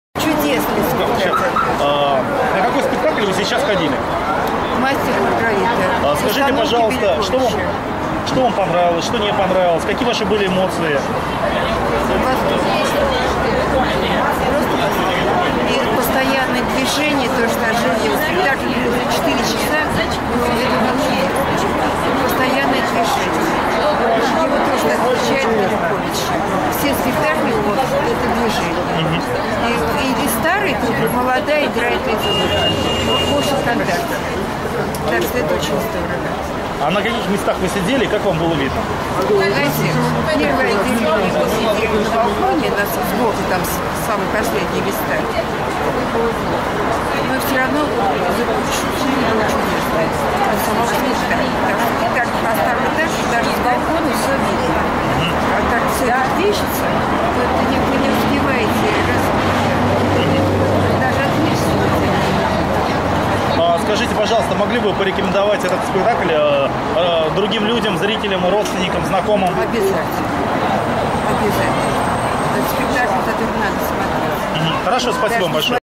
Мастер и Маргарита в театре Станиславского 30.06.2012
Отзывы о спектакле Мастер и Маргарита в театре Станиславского 30 июня 2012